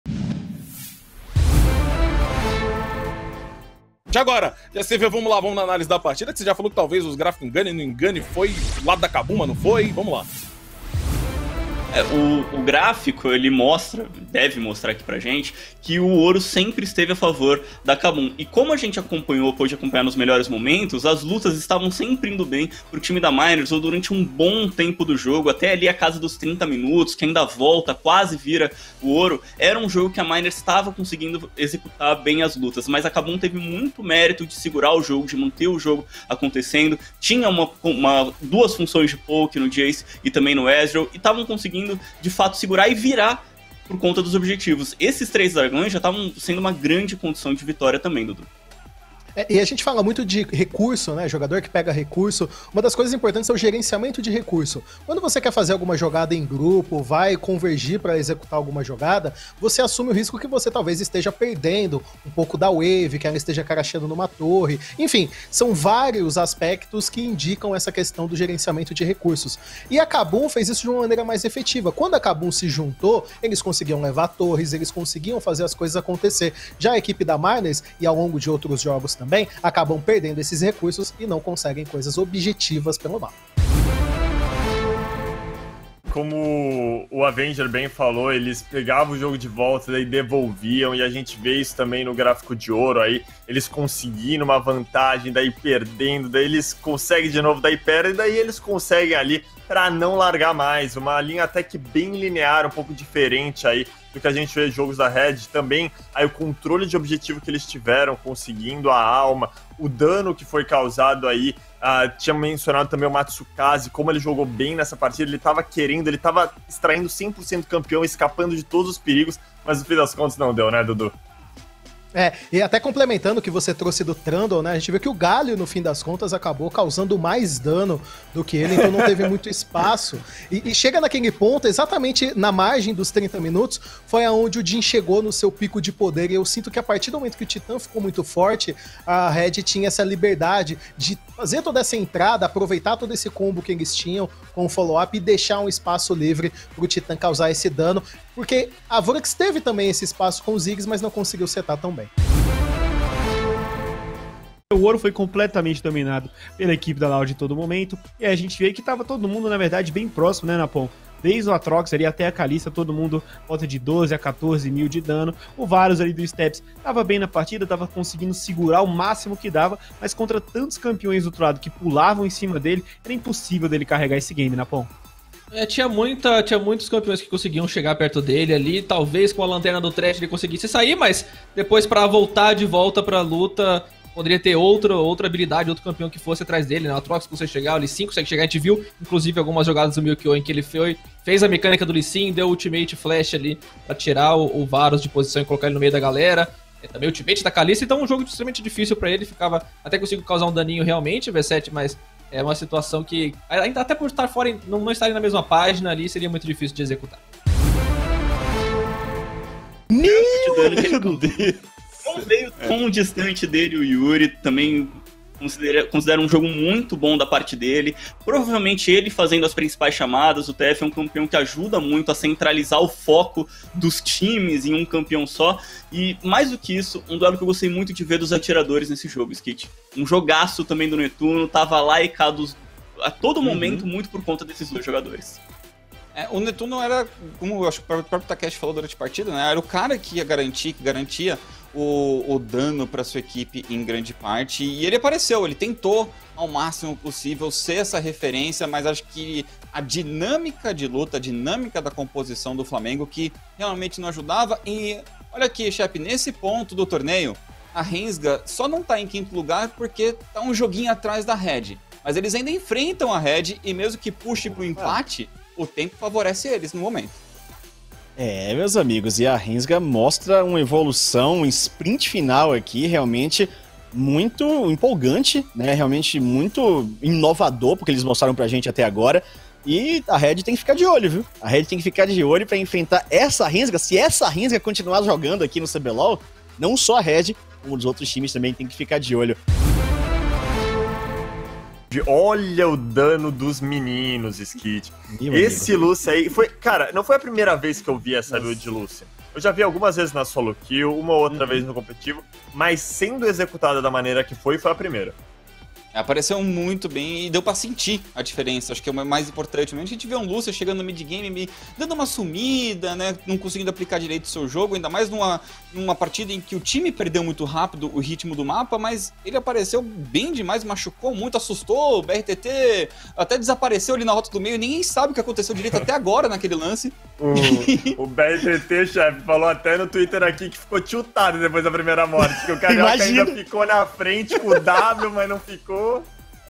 CBLOL 2021: 2ª Etapa - Rodada 16 | Análises das partidas
Veja em detalhes, com o olhar especializado dos nossos casters, como foram as partidas da décima sexta rodada do CBLOL, que acontece todos os sábados e domingos, a partir das 13h, nos nossos canais oficiais!